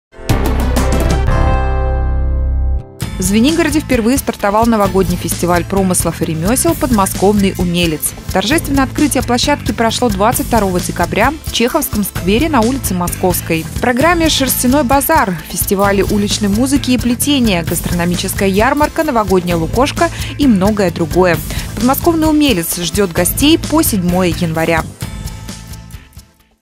Подмосковный Умелец
В Звенигороде впервые стартовал новогодний фестиваль промыслов и ремесел «Подмосковный Умелец».